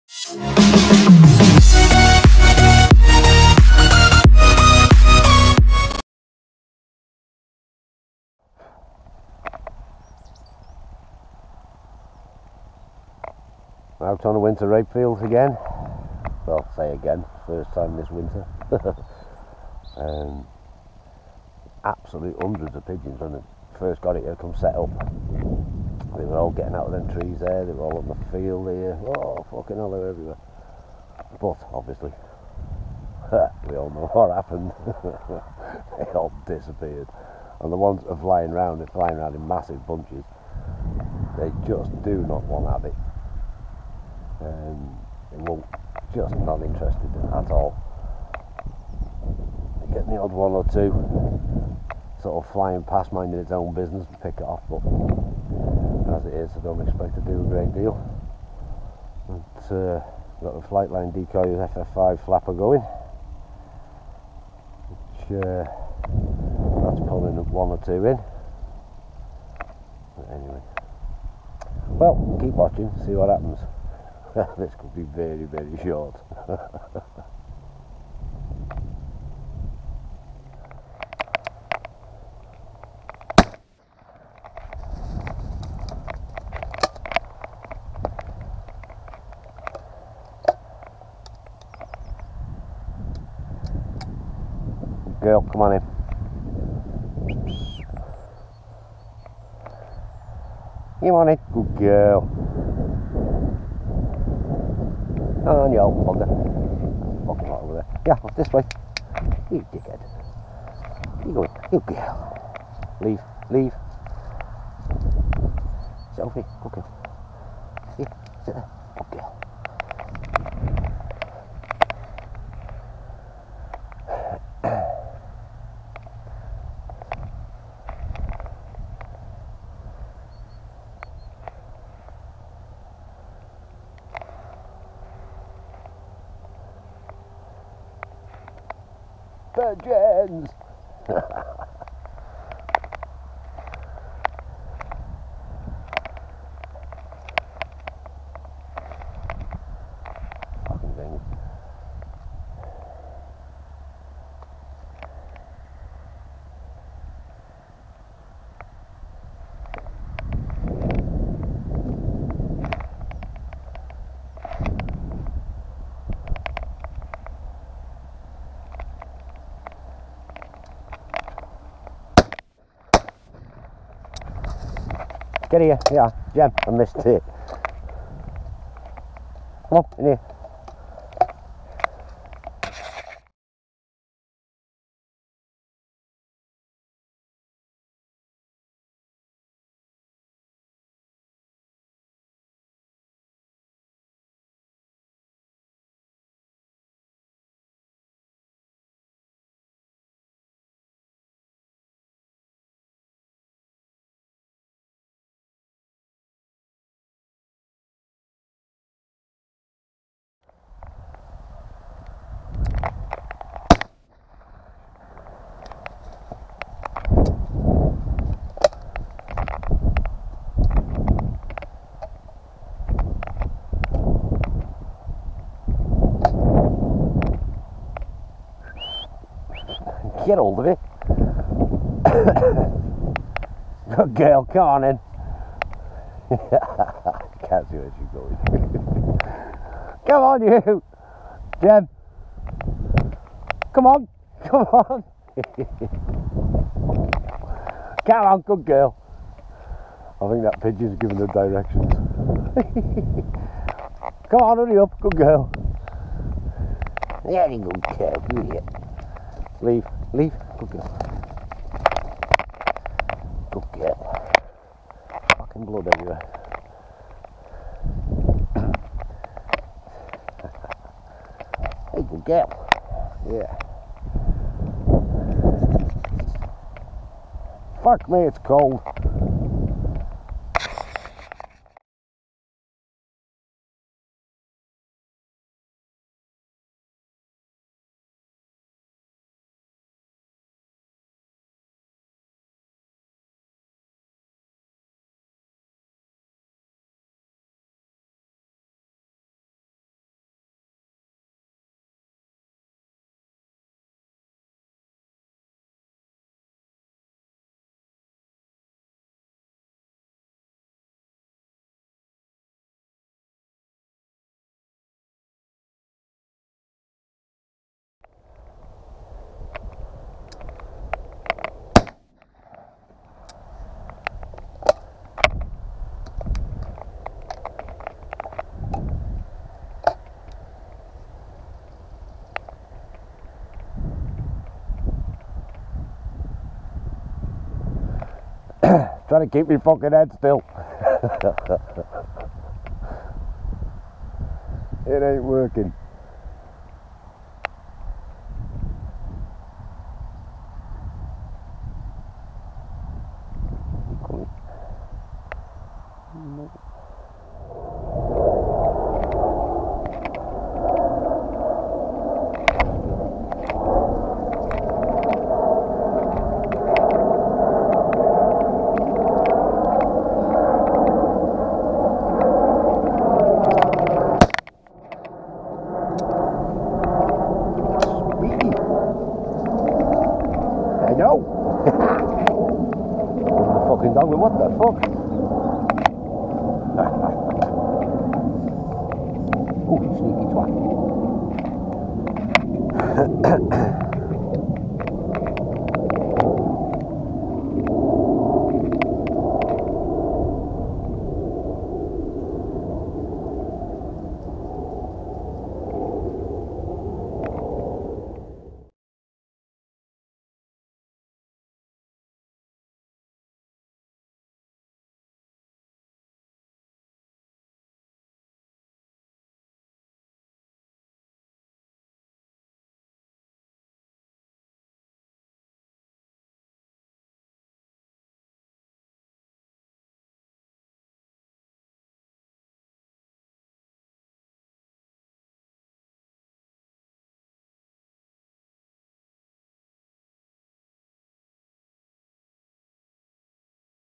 pigeon shooting over rape short video
quite a short video of some shooting over rape feb 5th 2018, birds would'nt decoy so only managed half a dozen